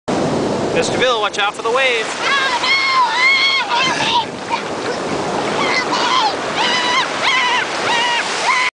Mr. Bill Gets hit by a wave
Mr. Bill thought he was going to have a nice day at the beach until he was taken out to sea.